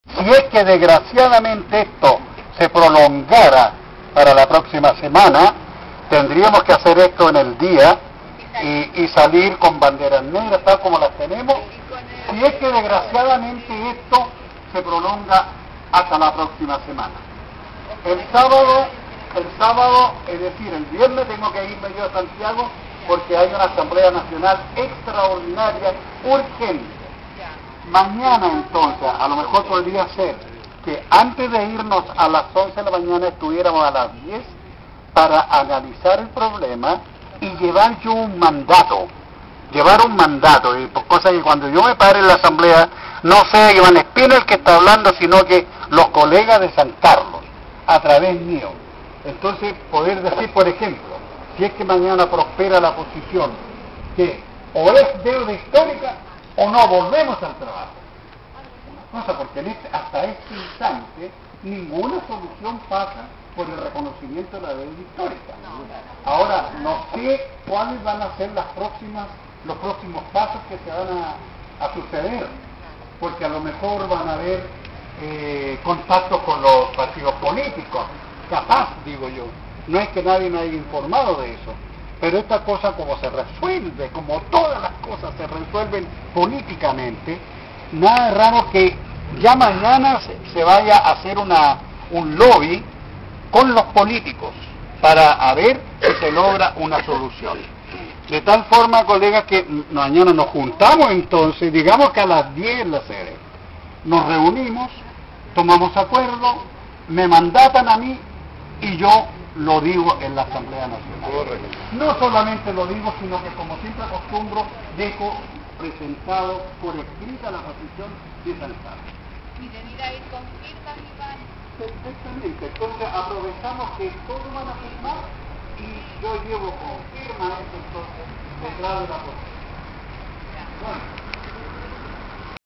Velatón por deuda histórica en San Carlos
Palabras del Presidente comunal, sr. Iván Espina. Lugar, plaza de armas de San Carlos, miercoles 28 de octubre, 2009
VIDEO: LGR